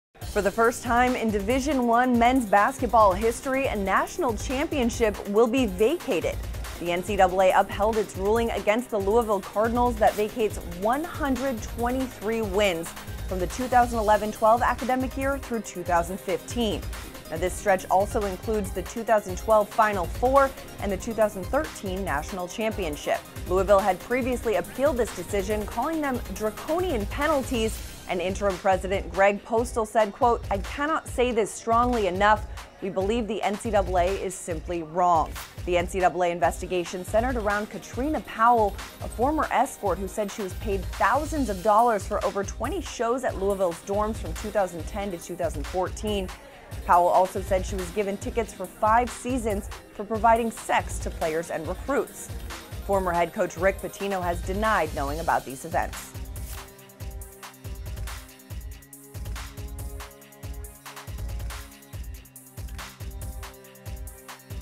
Louisville Ordered To Vacate 2013 Championship Amid Escort Scandal | SI Wire | Sports Illustrated
The NCAA upheld a ruling on the University of Louisville basketball case, ordering to school to vacate its 2013 championship amid an escort scandal.